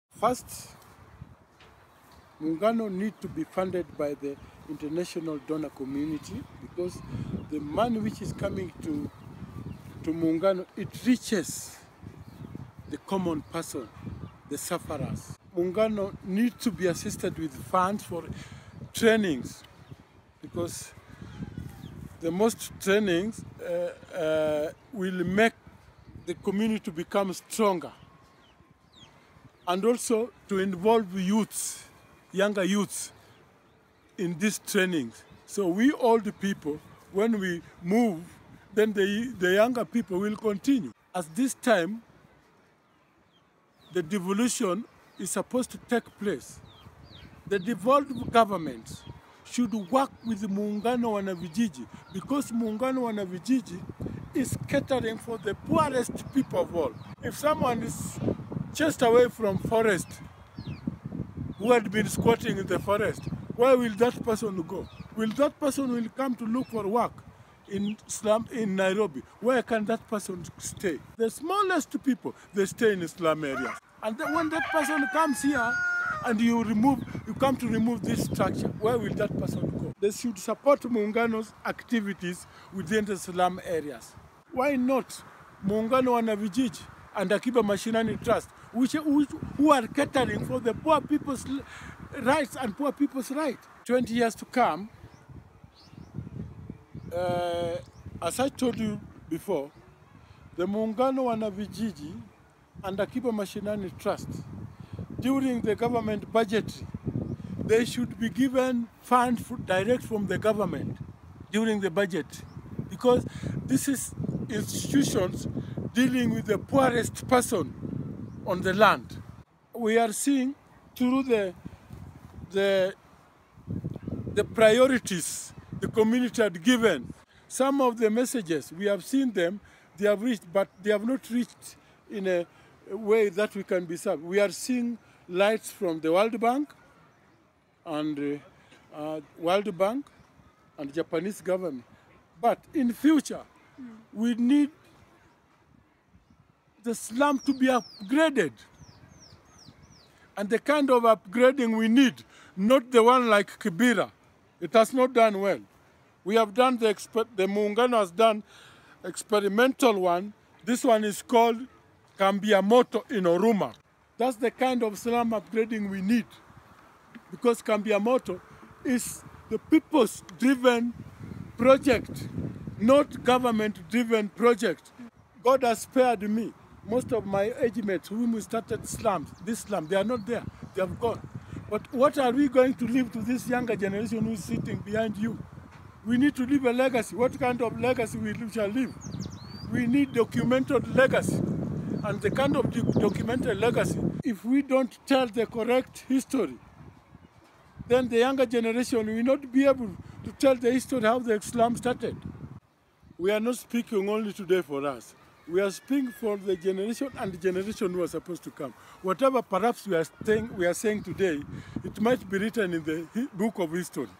Evans 'Papa' Omondi 5/5: What are your hopes for Muungano’s next 20 years?
This interview is with:

Evans Papa Omondi
Muungano
From: Mukuru kwa Reuben, Nairobi
Interview: 24 March 2016, Nairobi
Interviewer(s): Kate Lines
Original language: English